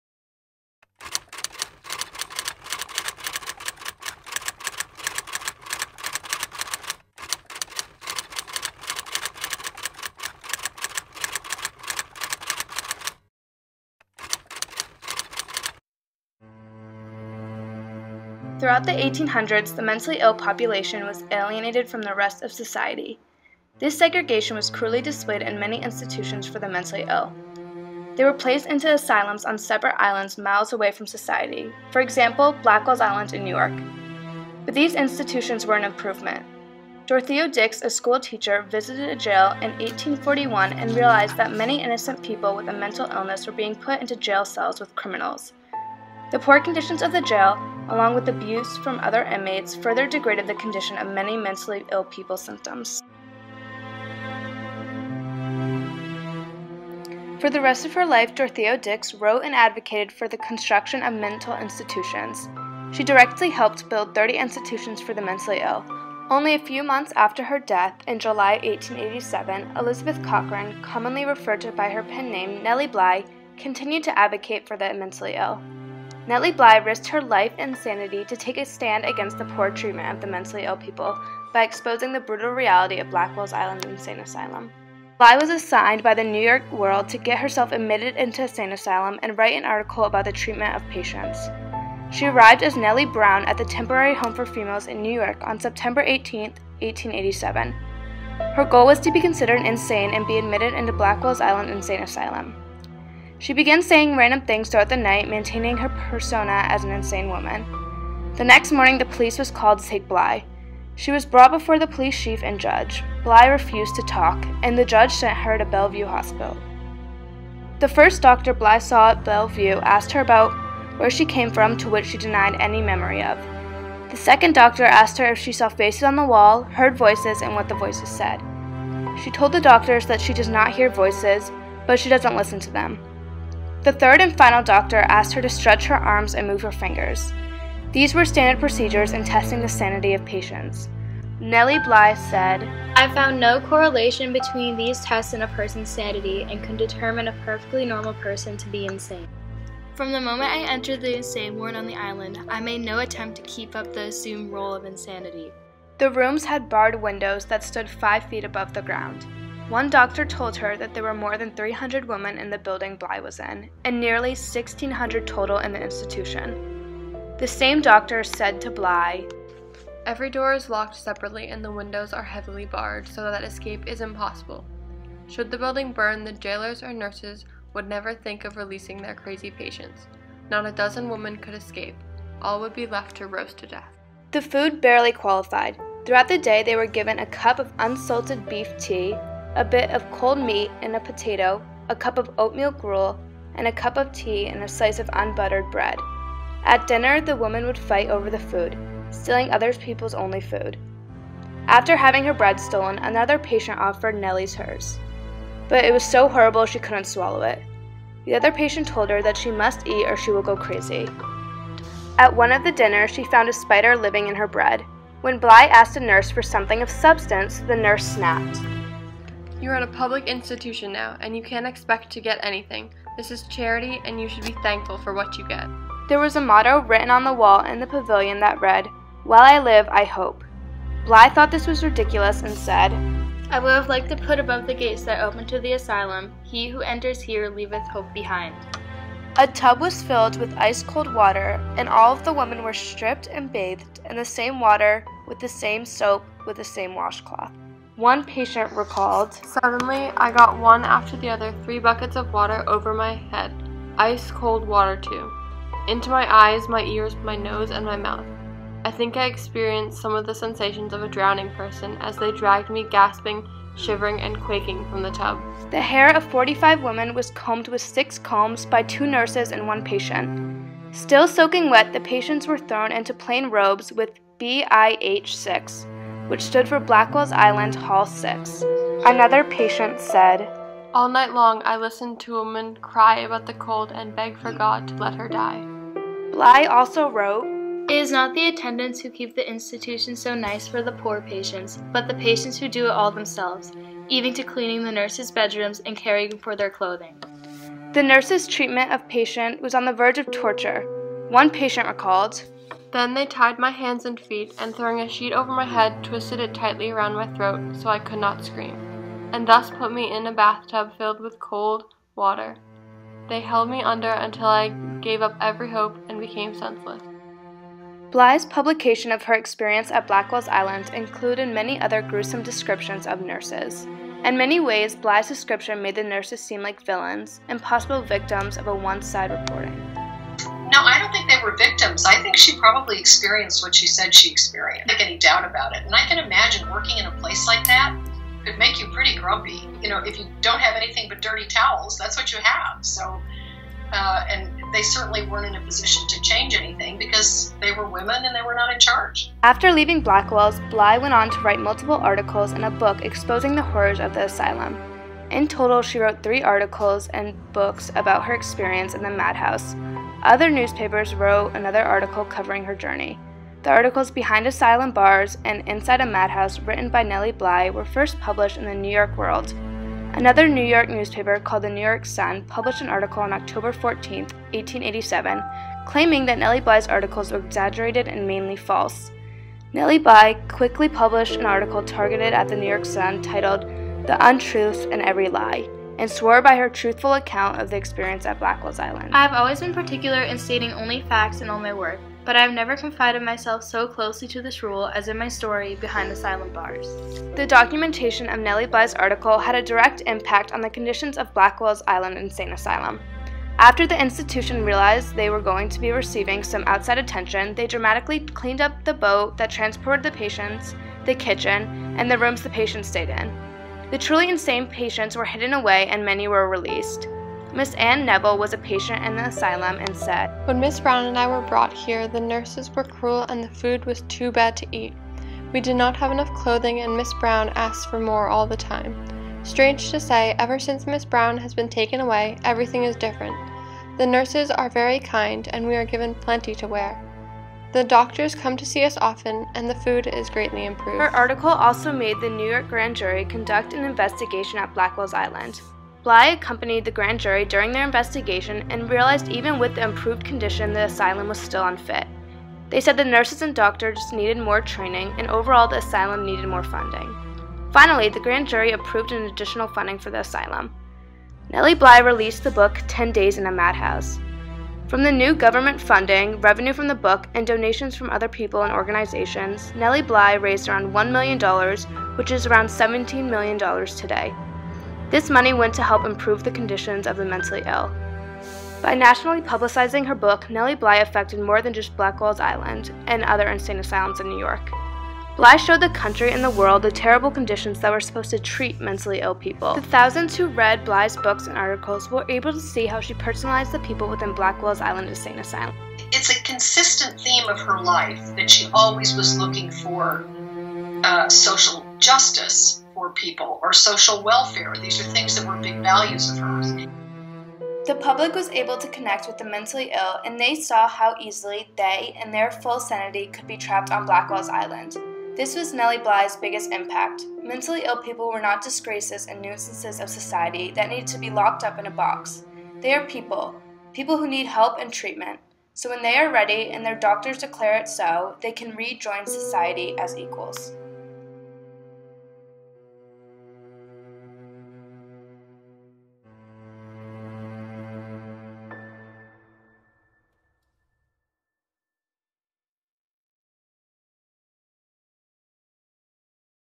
Journey Into the Madhouse: Nellie Bly's Ten Days of Insanity
National History Day Competition 2017
Documentary by Jessica Barker, Katherine Hoppler, Emerson McMannus